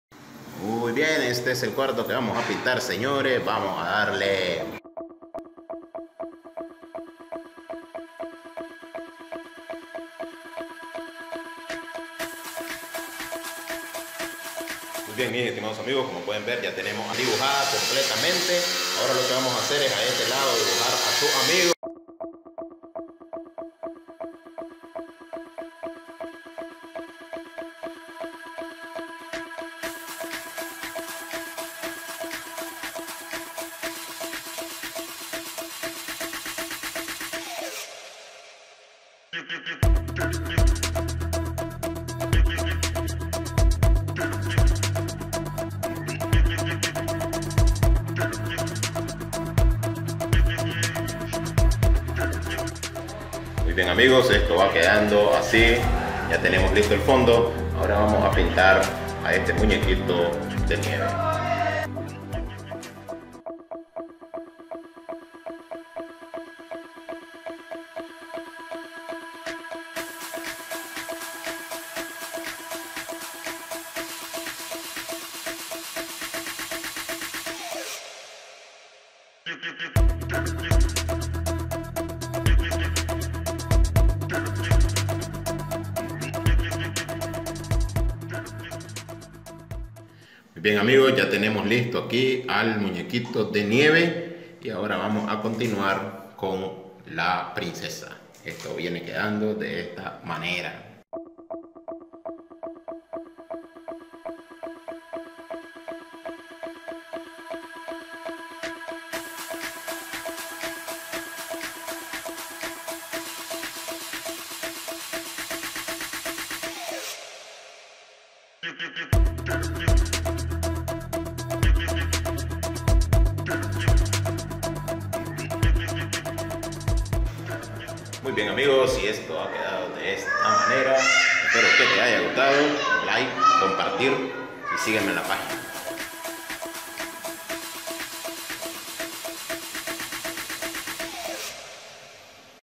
Pintando a Frozen y a Olaf en un cuarto
En este video te mostramos el trabajo que realizamos como artistas en recidencias.